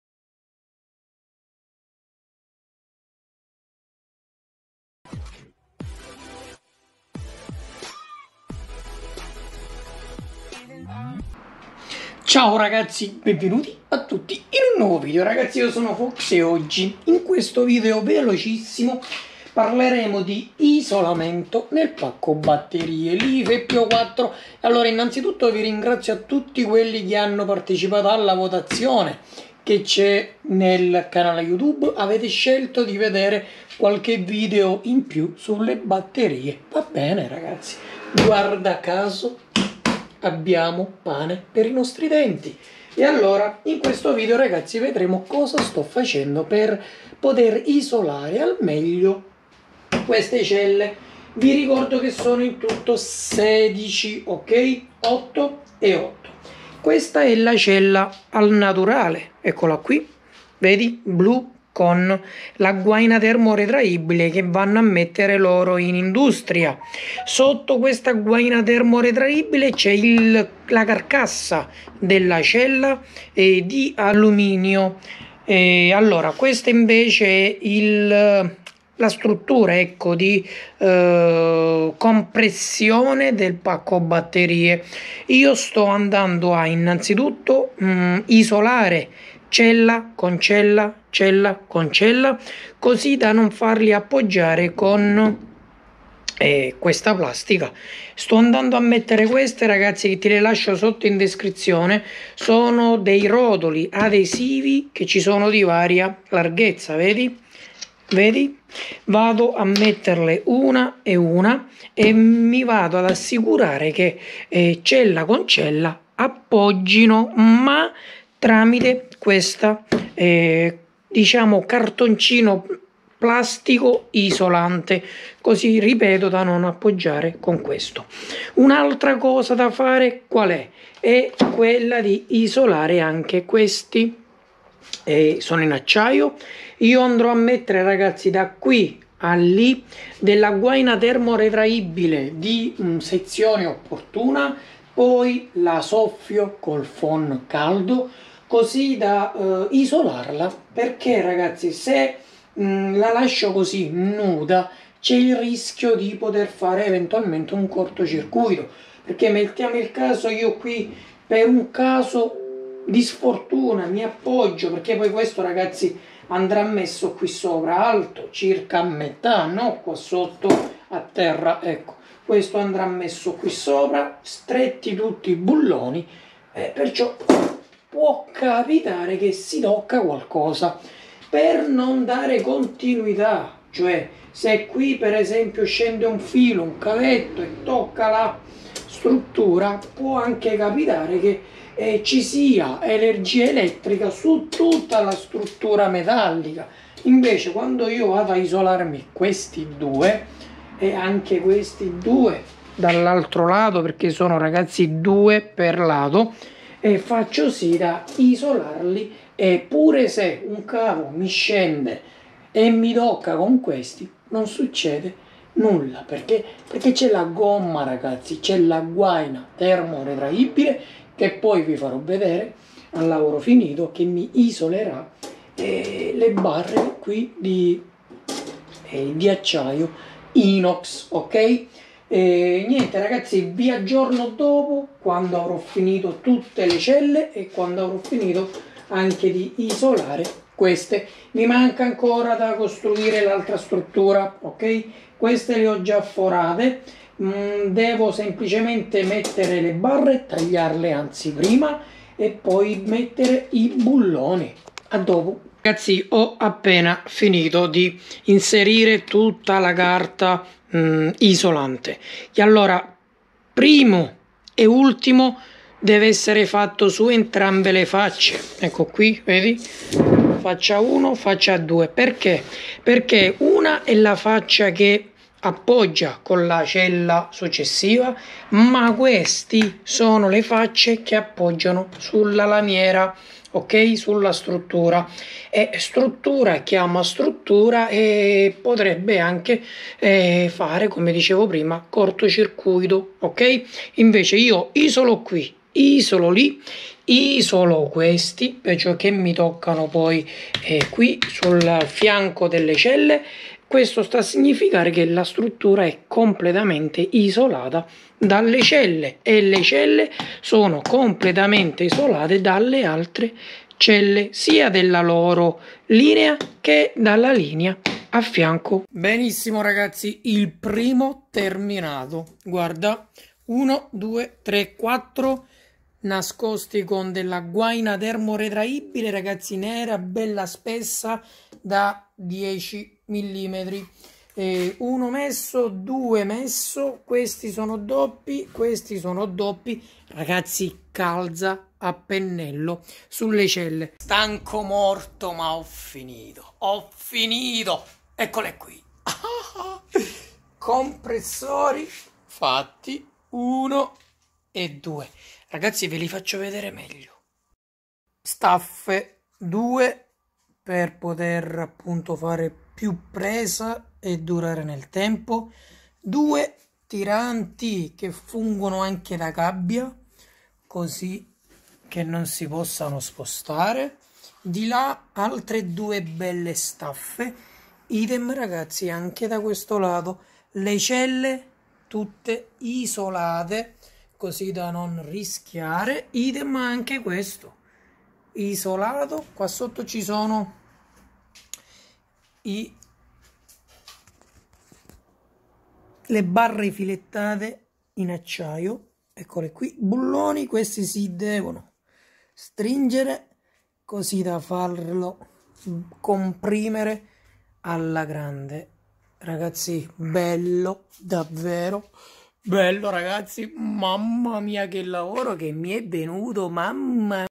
ISOLAMENTO E COMPRESSIONE PACCO BATTERIE 48V 16 CELLE LIFEPO4 295AH
Il modello della cella è Lifepo4 280ah REPT
VI TERRò AGGIORNATI SULL'ARRIVO DELLE BATTERIE E SULLA QUALITA

altre batterie e bms